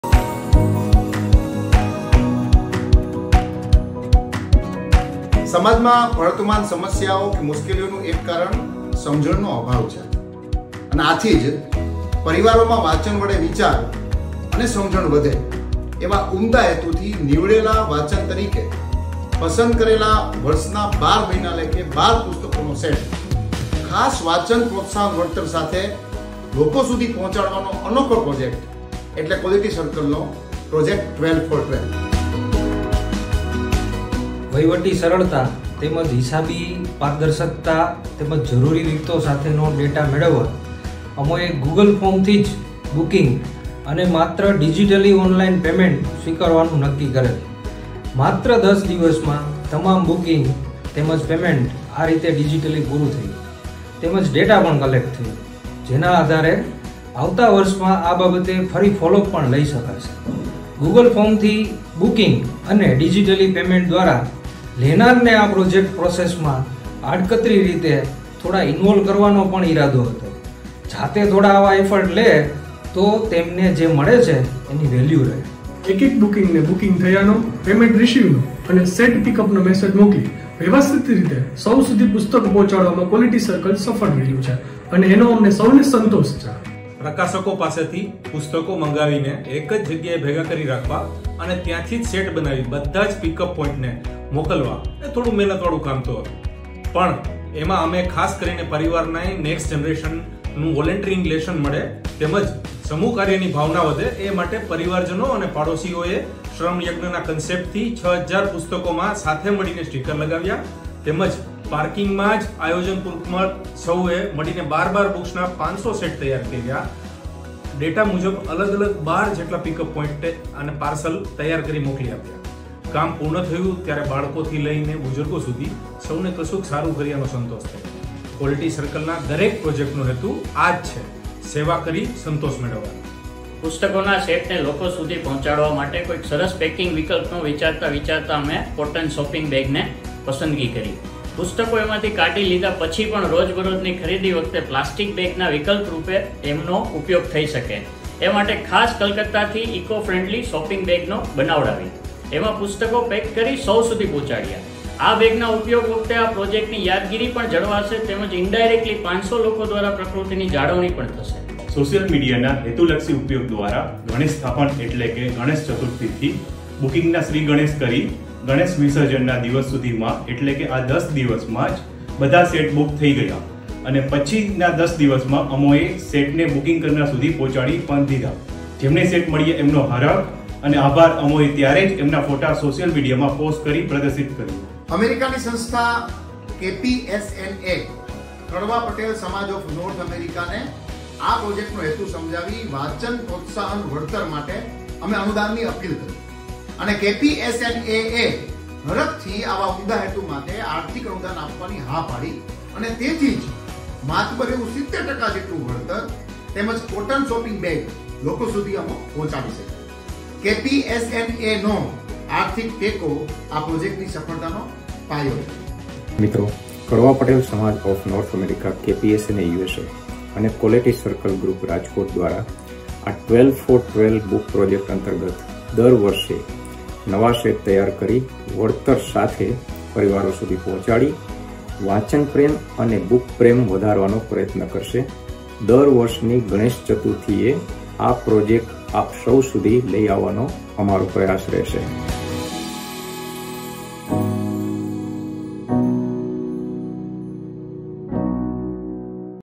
KPSNA's 12for12 Book Project
હું વાંચુ...આપ વાંચો...સૌ વાંચીએ

KPSNA-USA, in collaboration with Quality Circle, India, proudly presents the "12for12 Book" initiative, designed to promote reading awareness and a lifelong love for books. With a goal of 12 books for each of the 12 months, our mission is to rekindle the importance of reading, a value that has waned in recent decades due to the surge in social media usage. This decline in reading has given rise to societal issues, including reduced empathy and acceptance. Our carefully curated selection of 12 books spans diverse genres, encompassing motivation, inspiration, culture, spirituality, lifestyle, nature appreciation, love, culinary delights, and captivating children's stories. Join us in this transformative literary journey!